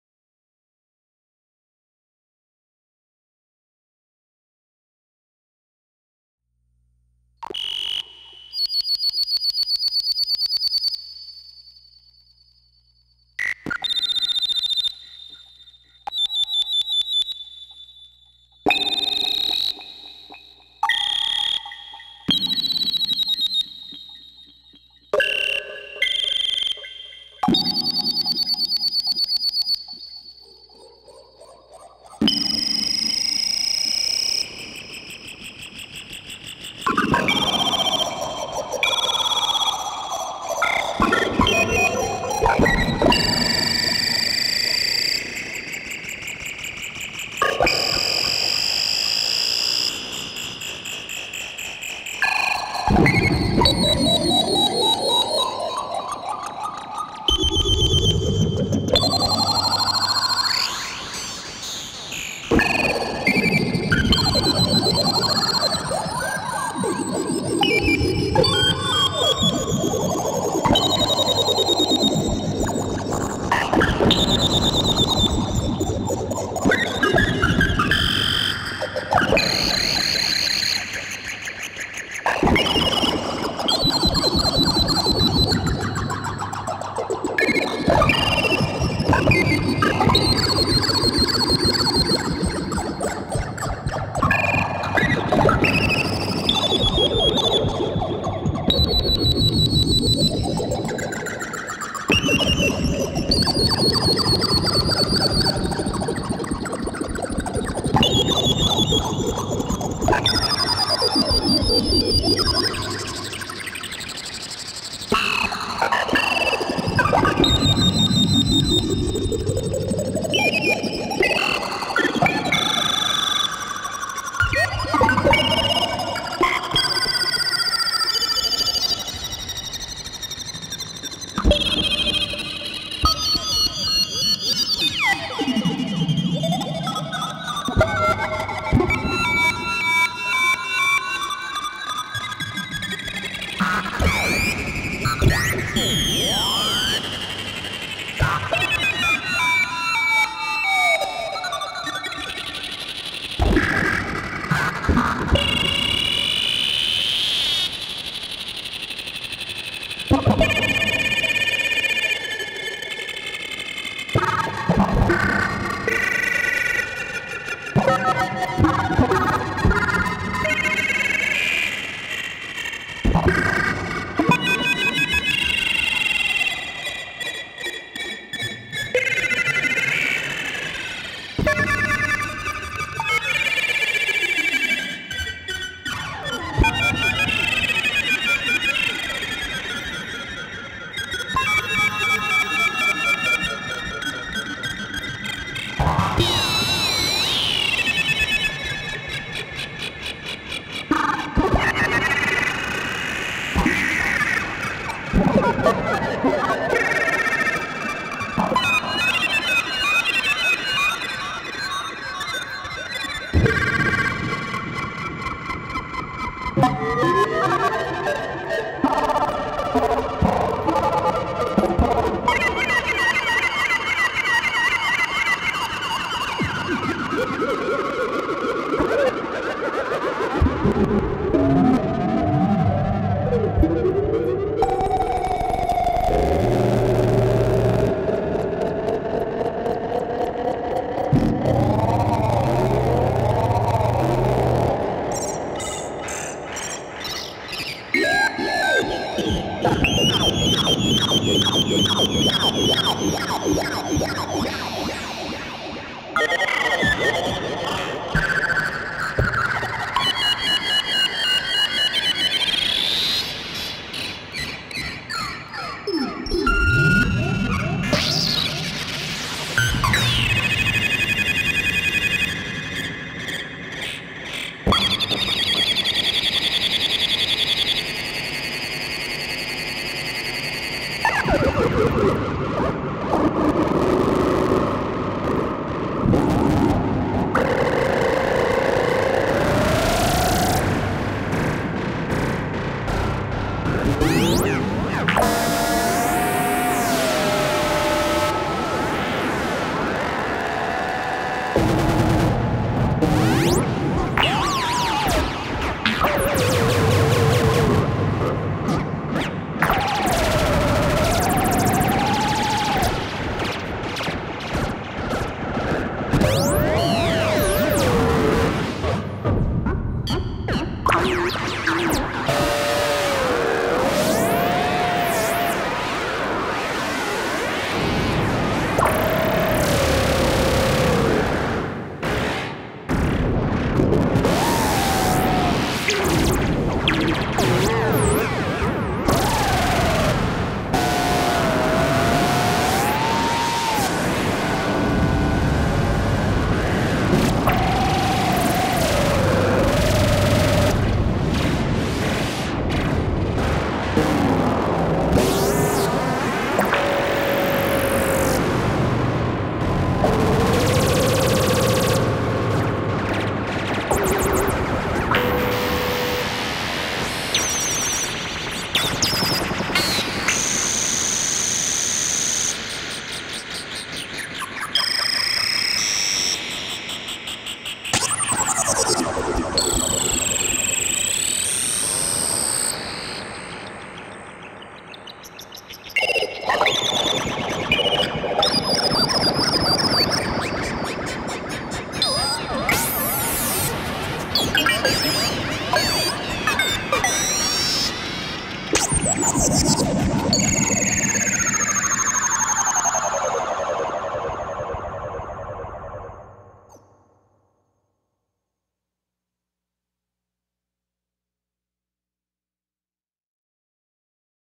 Patch for 3 Ring Modulators
For this patch I set up 2 ring mod sounds on the Synthesizers.com modular and the Behringer 2600 and sent them into the Erica Synths Syntrx 2's ring modulator. So ringmodding ringmod. I then sent the Syntrx joystick X and Y outs to the scope output and split/inverted it and sent it to the 2600 and dotcom. No Syntrx oscillators are being used here for audio.